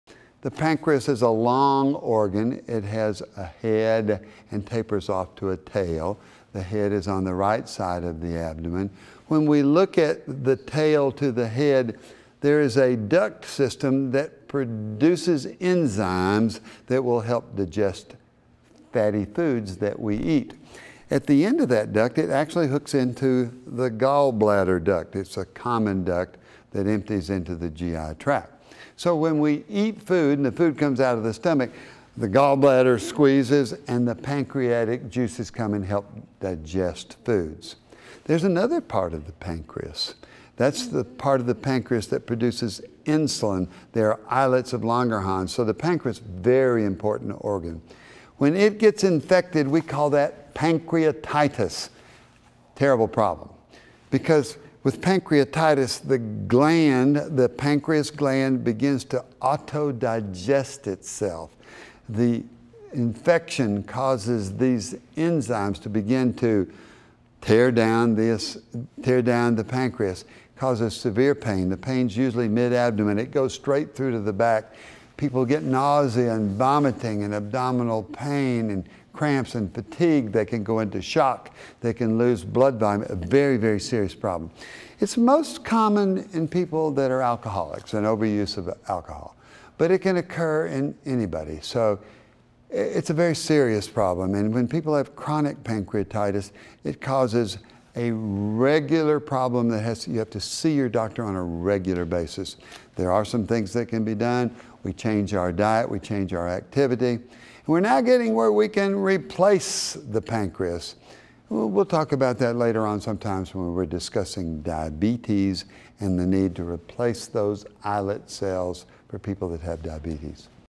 Pancreas Infection
Dr. Bob tells us the symptoms of an infected pancreas and how it's treated.